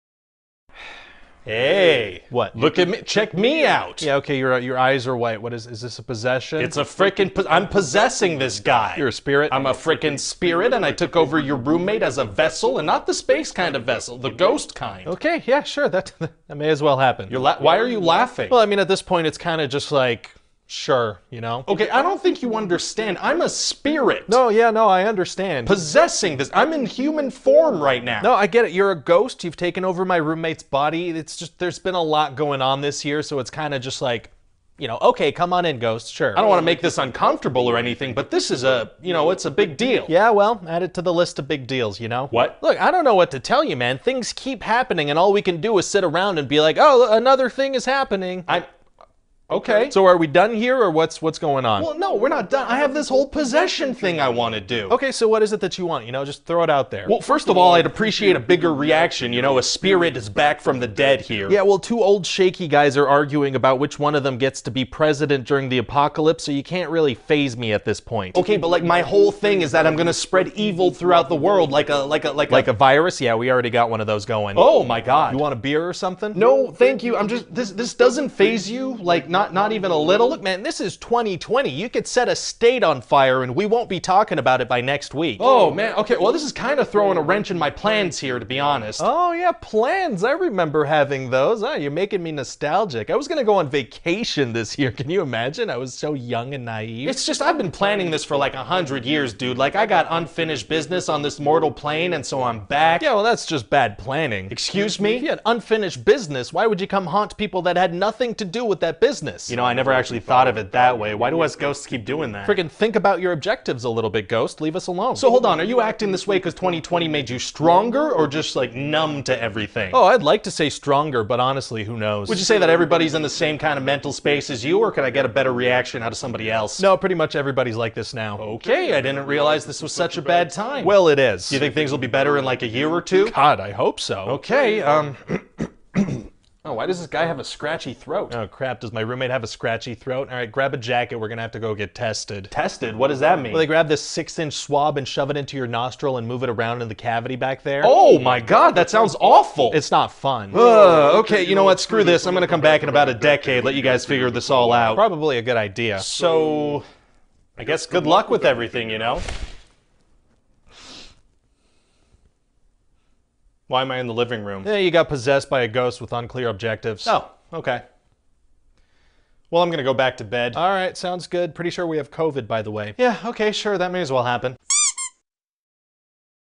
If Inferno possessed Addie in 2020 [crappost]
TW: 2020

I’ve had this in procreate dreams for like three weeks I kept forgetting to post it

This audio was sitting in my SCRATCH drafts, which is how I found it, my GOD I’ve wanted to do this for a while-

I swear I’m making actual content the content just isn’t contenting

OCs: Grace, Inferno, Addie

Time taken: an hour or twoish

Programs used:
Procreate Dreams - 10 FPS

Audio: forgor